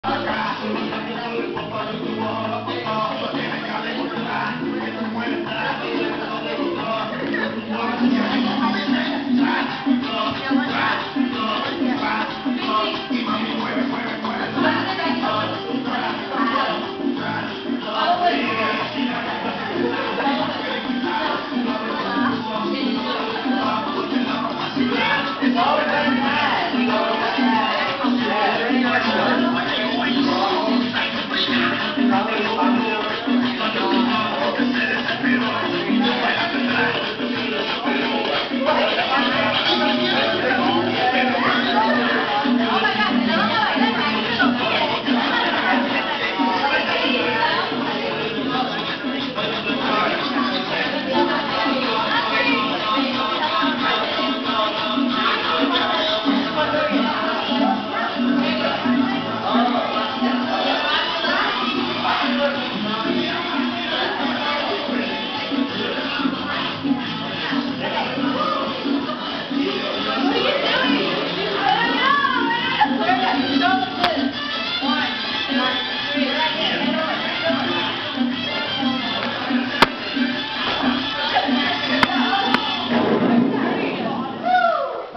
Mr. Clarksburg Latin dance rehersal
Mr. Clarksburg Latin dance rehersal Merengue

ugg sorry for the weird color glitches i dont know what happend.. my camera was being weird